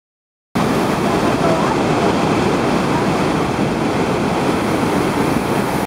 Cave of the Winds - structure
The decking is secured by wedging it between the rocks.  It's removed every winter when the falls freeze.